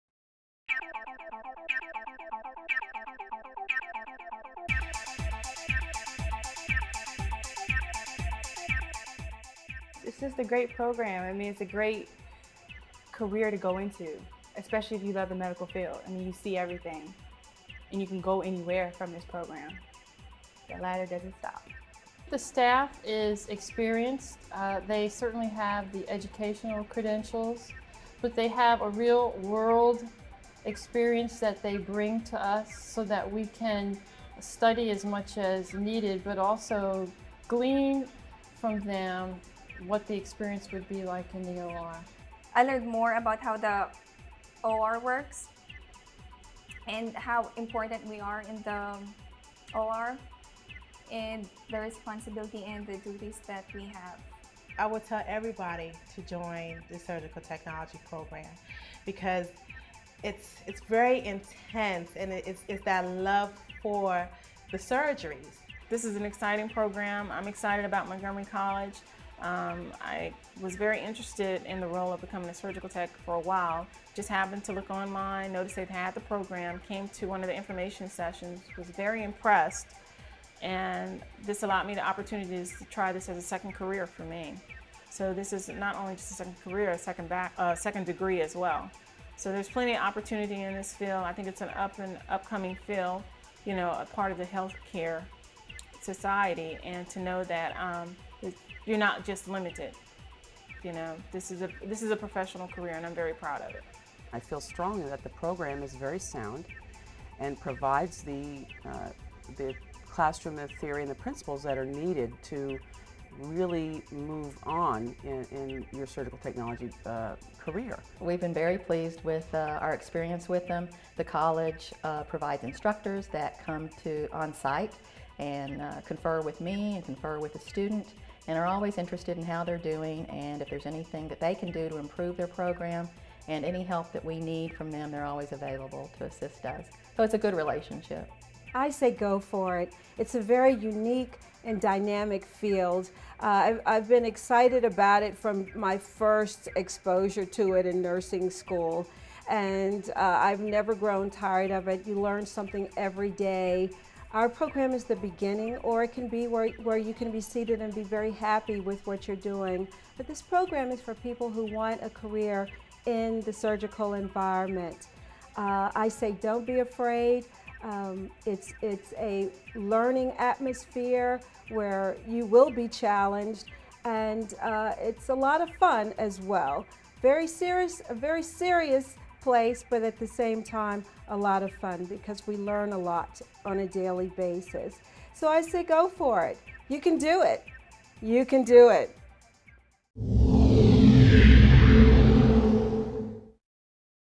Part 8 (of 8) "Making it Happen"
Surgical Technology:  A video to aid prospective students find out about Surgical Technology and what career options exist upon completion of the program at Montgomery College.  Segments include testimonials from former students, local healthcare professionals and surgeons.